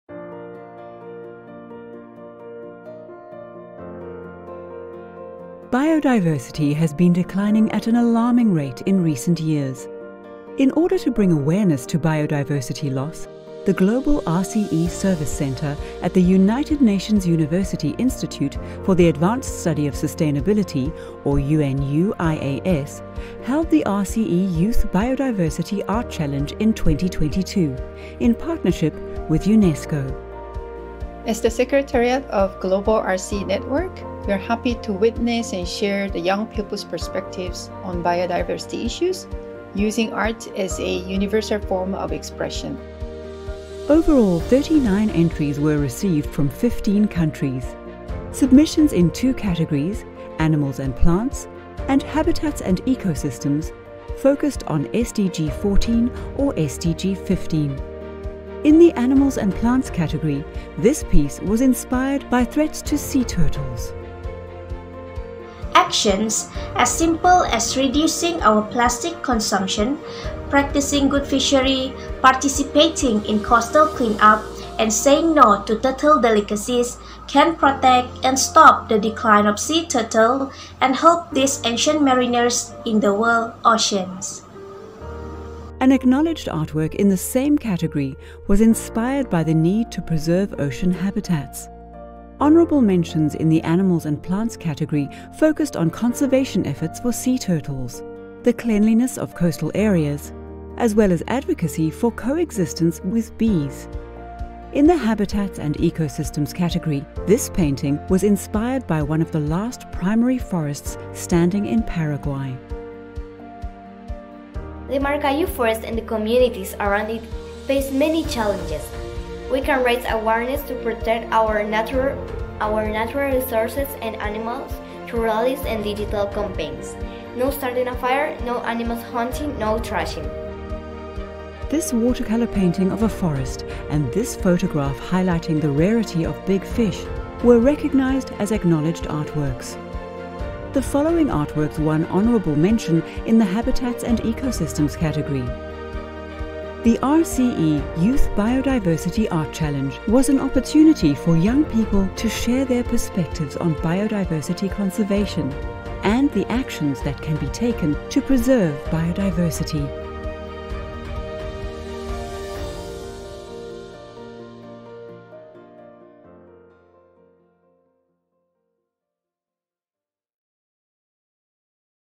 RCE Youth Biodiversity Art Challenge (shortened version)
The Global RCE Service Centre at the United Nations University Institute for the Advanced Study of Sustainability (UNU-IAS) is pleased to announce the awardees for the RCE Youth Biodiversity Art Challenge!

The challenge sought to put a spotlight on the species, habitats, and ecosystems that we need to work to protect, and asked participants to create works of art with a focus on SDG 14 (life below water) or SDG 15 (life on land).

Submissions were received across two categories (Animals & Plants and Habitats & Ecosystems), with 39 entries coming from 15 countries, from participants aged under 15 through to 35 years of age. Artworks were expressed through a range of mediums, including drawings, paintings, craft work, digital art, and photographs.